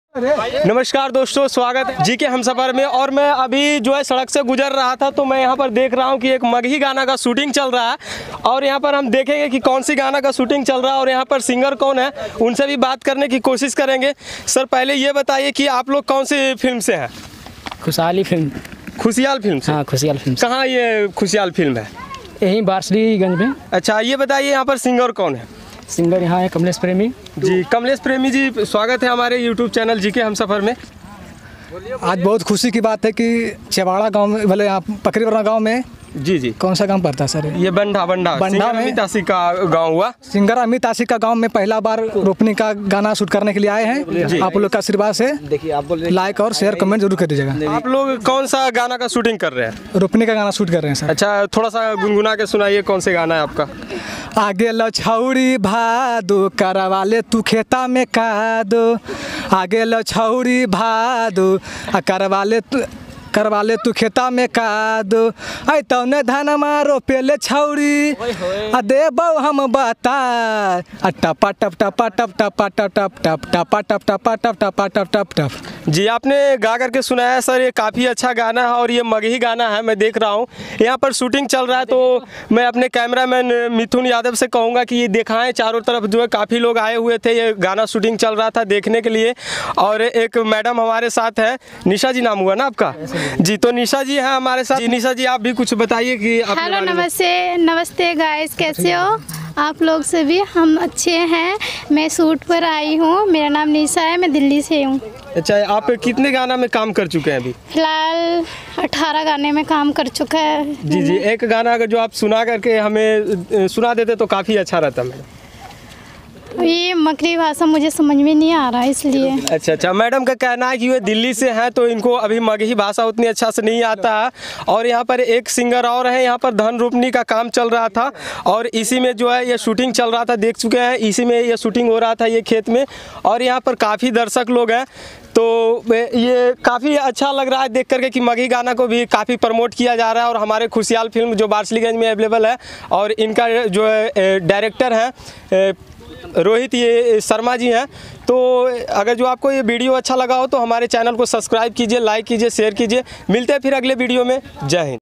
मगही सिंगर कमलेश प्रेमी खास बातचीत !! मगही सिंगर के इंटरव्यू !!
Maggi singer Kamlesh Premi se Khas batchit is interview ke Madhyam se yah dikhaya Gaya Hai Chhote kalakar ke Jivan Mein kitna ashrugan hota hai is Samay health ki avashyakta hoti hai use Samay Milta Nahin